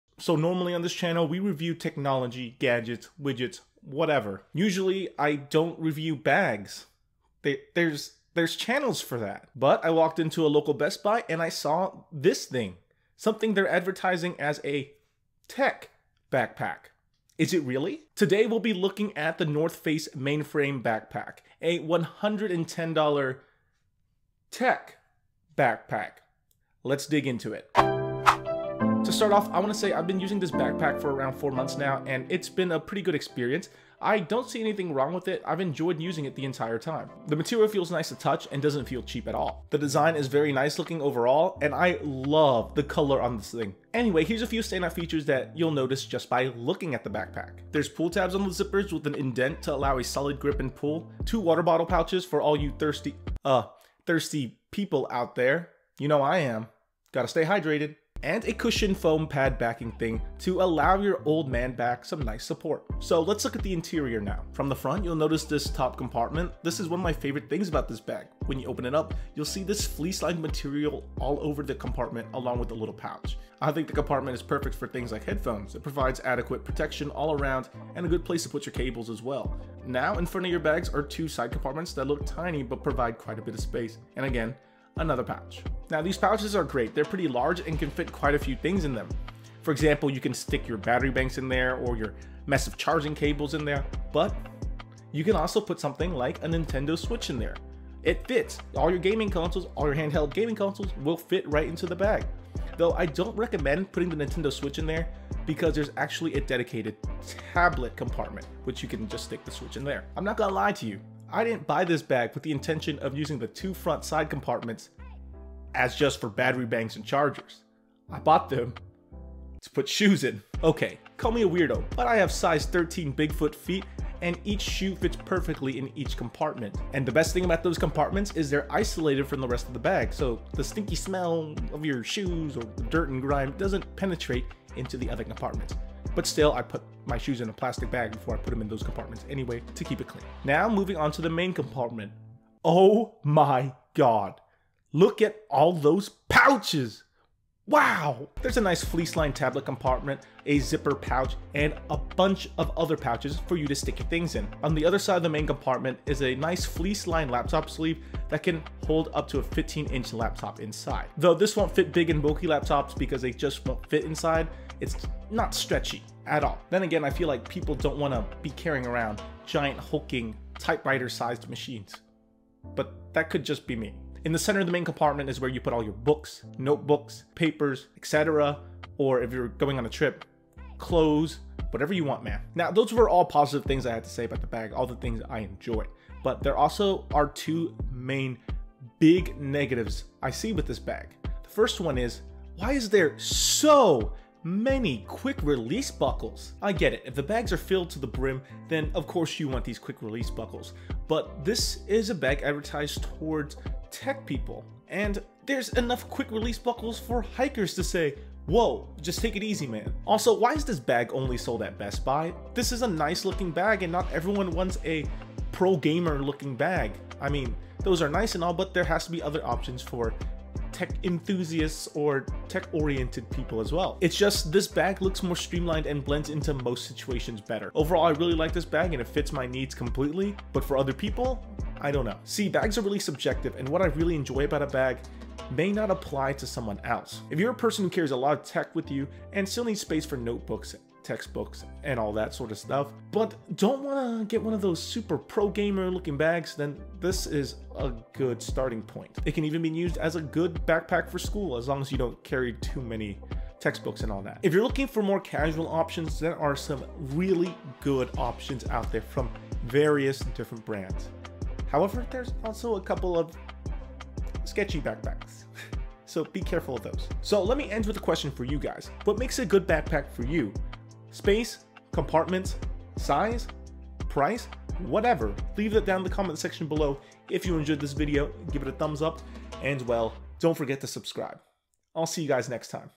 The North Face Mainframe: A TECH Backpack??
So normally, on this channel, we look at technology, gadgets, and widgets, whatever. Usually, I don’t review bags. There are channels for that. But when I walked into a local best buy, and saw this thing. They advertised as a “tech” backpack. Is it really? Today we’ll be looking at the North Face Mainframe backpack a $110 tech backpack. Let’s get started.

To start off, I want to say, I’ve been using this backpack for a good 4 months and I can tell you it’s a great experience. The material feels nice to touch and doesn’t feel cheap. The design is very nice looking overall, and I love the color on the thing. It’s cool. WOW. Anyway. Here’s a few standout features you’ll notice just by looking at the backpack. There’s pull tabs on the zippers, with an indent to allow a solid grip and pull, two water bottle pouches, for all you thirsty uhh.. thirsty people out there. You know I am. Gotta stay hydrated. And a cushion foam pad backing thing to allow your aching old man back some nice support.

So, let’s talk about the interior now. From the front you’ll notice this top compartment, this is one of my favorite things about this bag, when you open it up you’ll see this fleece lined material all over the compartment along with a little pouch. I think the compartment is perfect for headphones, it provides adequate protection all around and a good place to put your cables as well. Since it is soft shell, I wouldn’t recommend putting super delicate things here like camera equipment, that’s a huge mess waiting to happen. Now, in front of the bag are two side compartments that look tiny but provide quite a bit of space and again another pouch. Now this pouch is great, it can fit things that are pretty large. I’d say these compartments are great for your battery banks, large mess of charging cables, or even, check this out, your handheld game consoles, like a Nintendo switch. But I wouldn’t recommend putting a switch there because there is a dedicated tablet compartment. I’m not gonna lie, I didn’t buy this backpack with the intention of using the side compartments as “cable, battery, and console” storage, but more of storage for shoes. Call me a weirdo but I can tell you, one shoe on each side? With my size 13 bigfoot feet. It fits and is isolated from the main compartment. Which is nice. Now onto the main compartment. Look at, all those, pouches. Wow. There’s a nice fleece lined built in tablet compartment, a zipper pocket, and so many other little pockets to stick your things in. On the other side there’s a fleece lined laptop compartment that can fit up to a 15in laptop. It’s not super stretchy, so I don’t think it’ll fit thick laptops, even smaller ones a little hard to take out of the compartment because it’s pretty tight. In the center of the main compartment is where everything else goes, like notebooks, books, papers, whatever. If you’re on a trip. Clothes. Etc. Whatever you want man.

Now those are all great things about this bag, I really enjoy it. But there are a few of negatives as well. For one, there are, for some reason, sooooooo many quick release buckles. I get it, if the bag is filled to the brim with too much stuff, then this would help, but this is a bag marketed for tech people, there’s no need for the crazy amounts that even Hikers would say, “woah, just take it easy man”. Also, why is this bag only sold at best buy? It’s a nice bag and fits a large demographic. Not everyone wants “pro gamer” bags. It’s just… no. This looks much more streamlined and blends right into any situation. But really, those my only complaints about this bag.

Overall, I really like this bag, it fits all of my own personal need. But for other people? I don’t know. See, bags are really subjective, and what I really enjoy about it may be completely different for another person. If you’re a person who carries lots of tech with you and still need room for other things, and like the general aesthetics of this bag go for it. I think it can be even used well as a casual bag for things like school, as long as you don’t carry too many papers or textbooks. If you were looking for a more casual options, there’s plenty of good backpacks from some reputable brands that could make a decent “tech-focused” backpack, and some uhh. Sketchier ones as well.

So, let me end with a question for you guys, what makes a good backpack for you? Space? Compartments? Price? OR whatever. Leave that down in the comment section below.